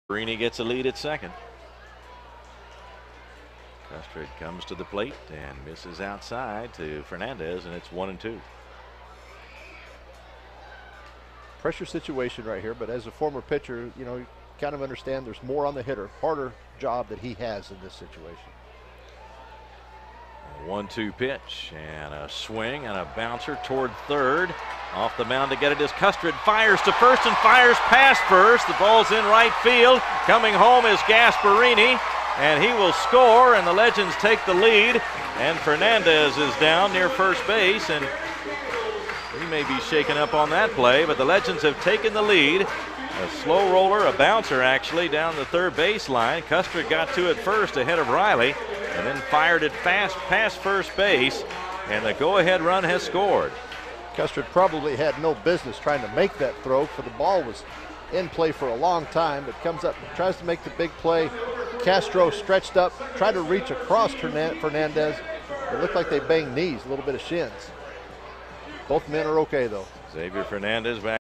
Legends take the lead! 2-1 Bot of 8th
Lexington Legends Baseball vs. Rome #3 - AJ Puckett debut